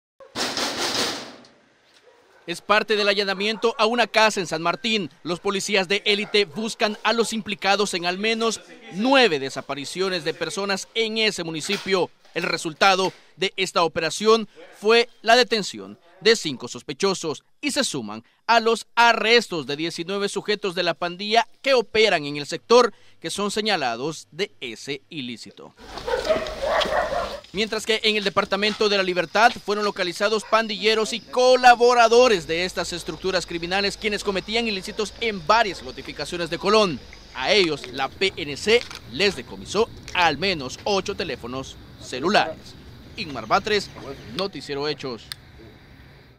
Operativos en San Martín y Colón
Un operativo de la PNC en algunos municipios de La Libertad y San Salvador, tenía como objetivo desarticular grupos de pandillas.